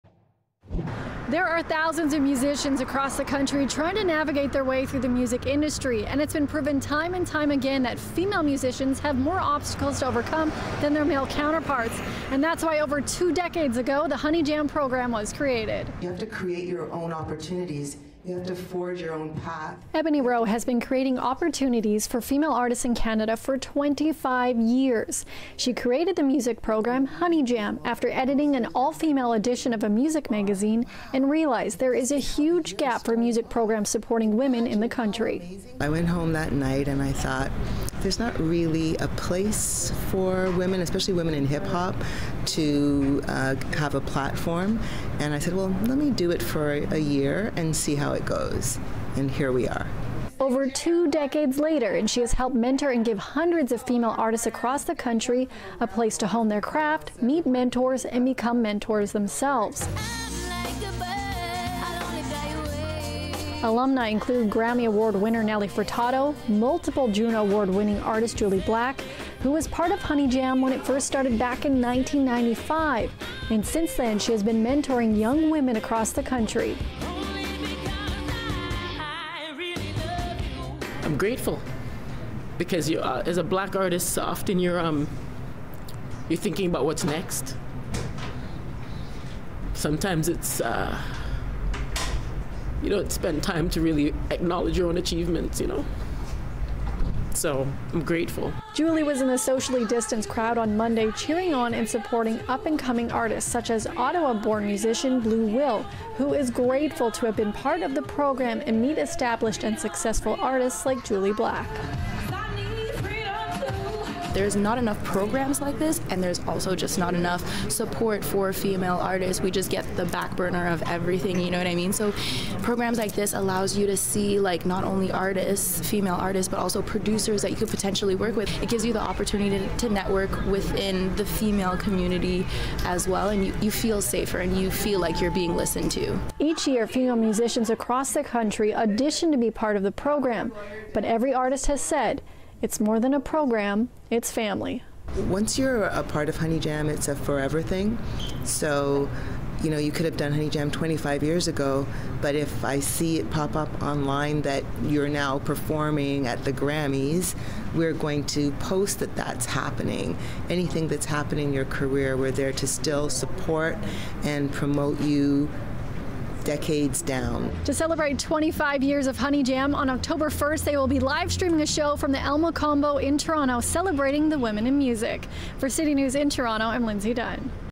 Honey Jam: fostering female artists for 25 years
A music program in Toronto has been fostering female artists across the country for 25 years. Lindsay Dunn speaks to the creator of Honey Jam and program alumni Jully Black about its impact on the music community.